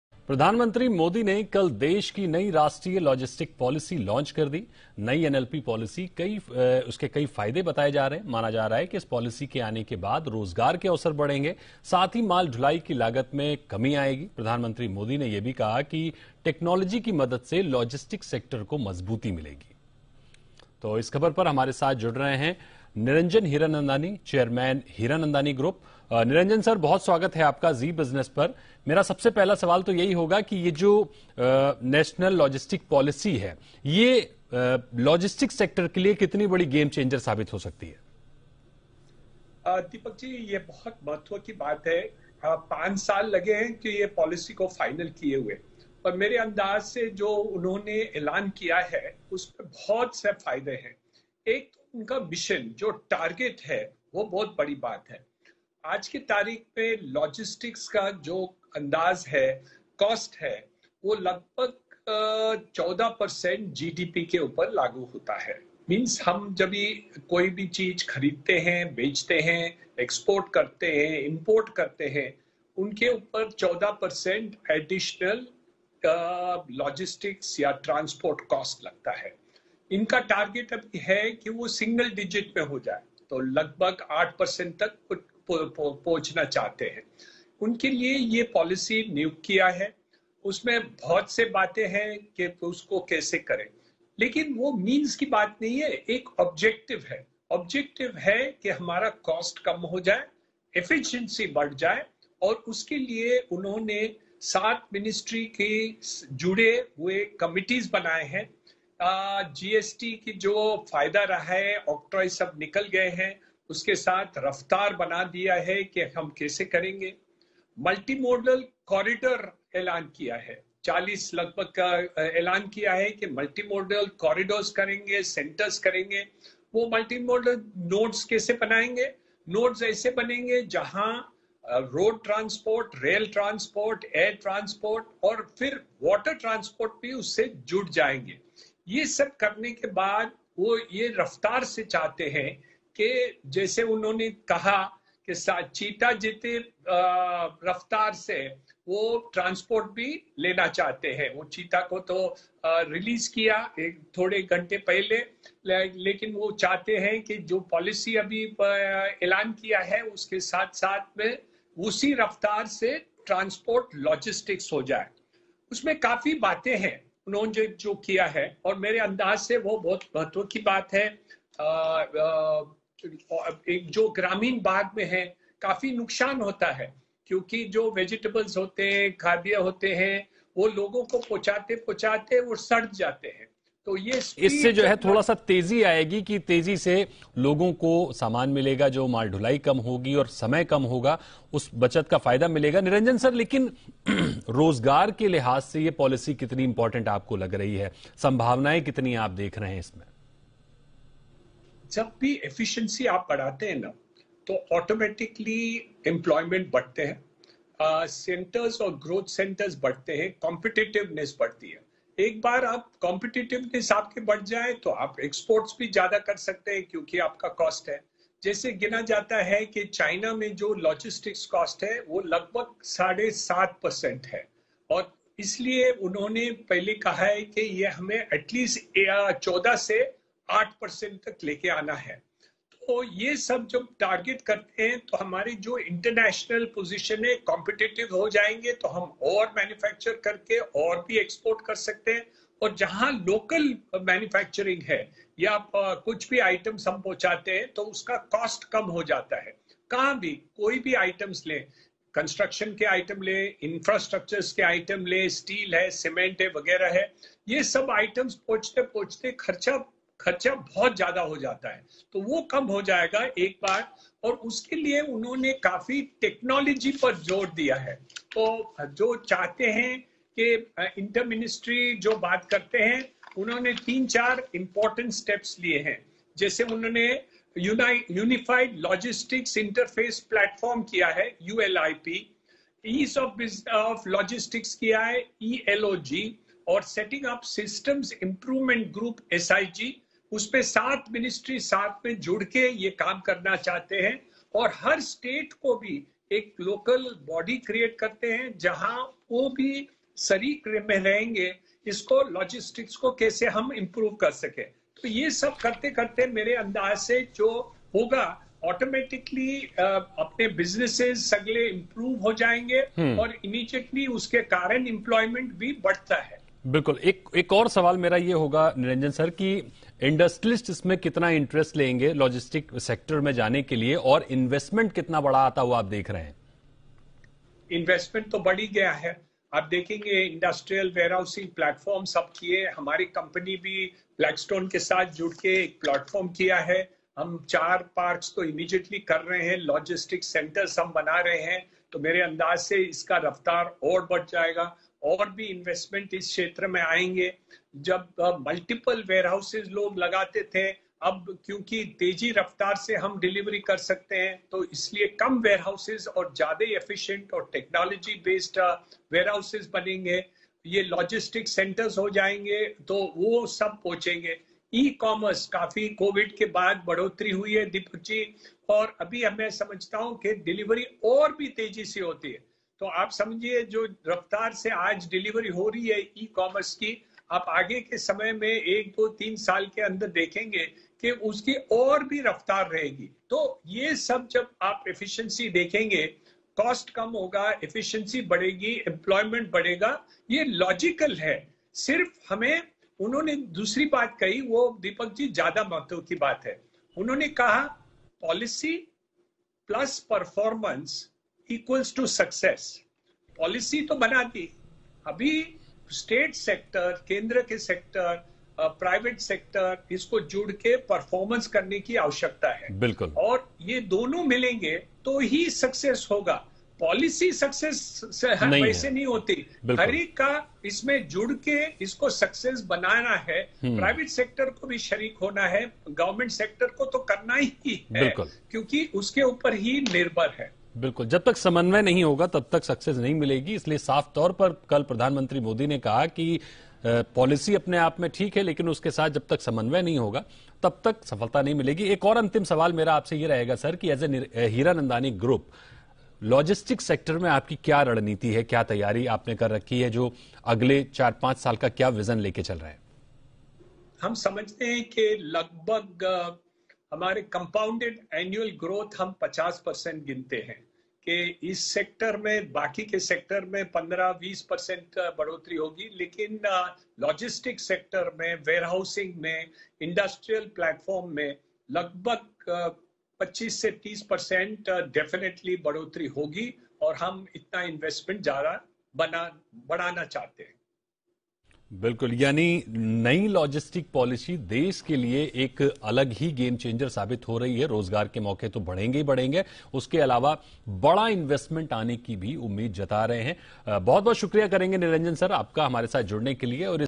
Niranjan Hiranandani, Chairman, Hiranandani Group discusses New National Logistic Policy
PMModi ने कल देश की नई राष्ट्रीय लॉजिस्टिक्स पॉलिसी लॉन्च की

NationalLogisticsPolicy लॉजिस्टिक सेक्टर के लिए कितना बड़ा गेम चेंजिंग साबित होगा?

 जानिए इसपर हीरानंदानी ग्रुप के चैयरमैन निरंजन हीरानंदानी का क्या है कहना...